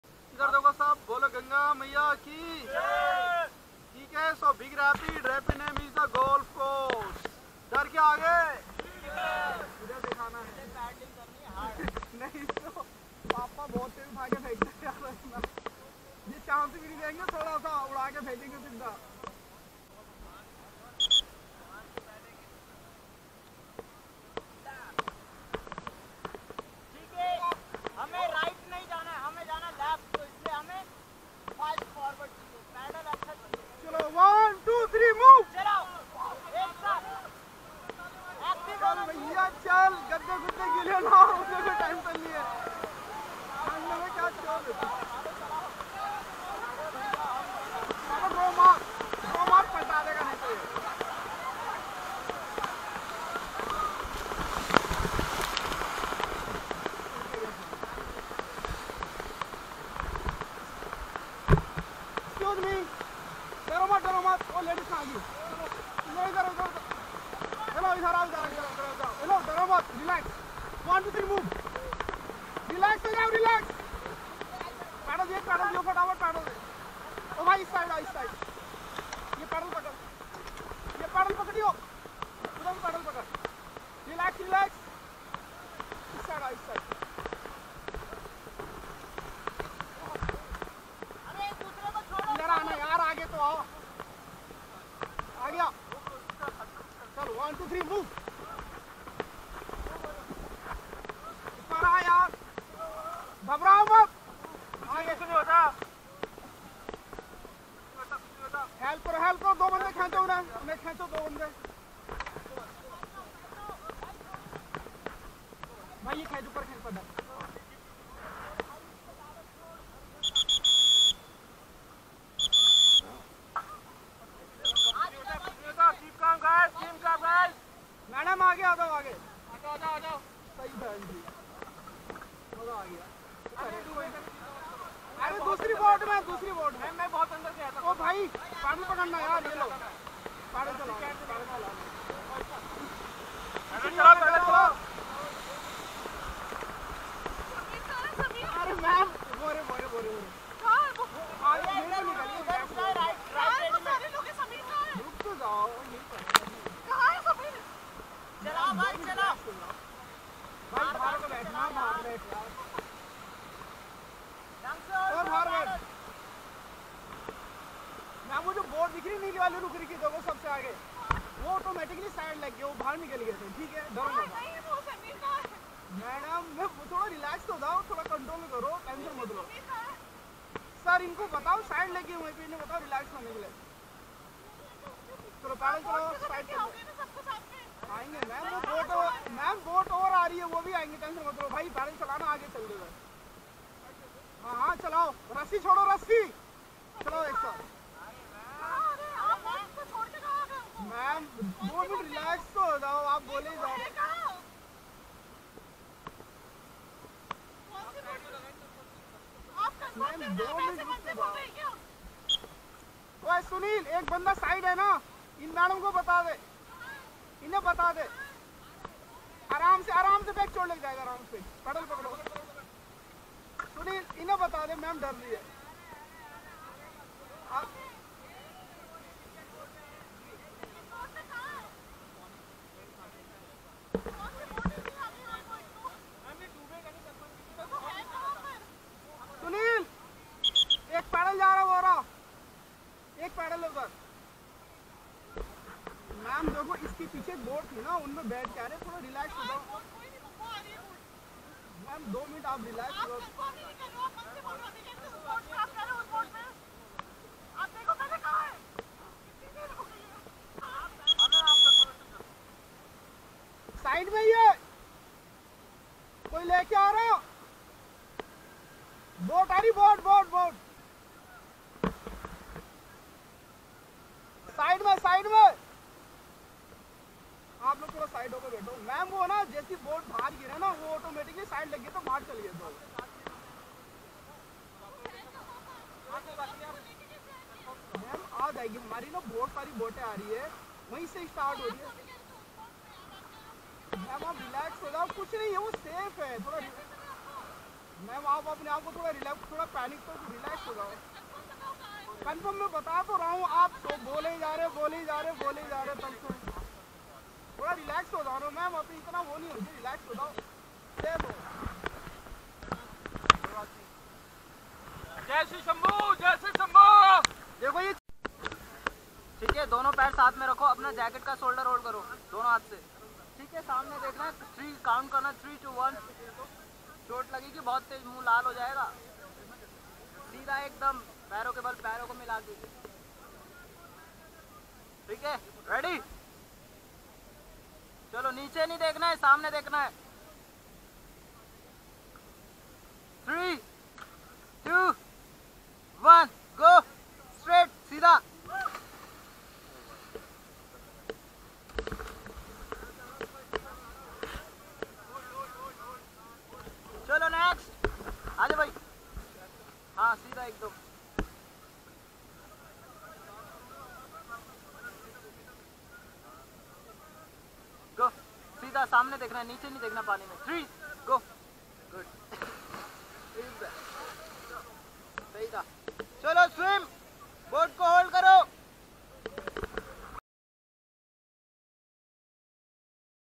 One Person Left in River! Rishikesh river rafting accident
This was one of the best things you could do in rishikesh ...we can't forget the moment when we all fell into the river.. part 2 will be coming soon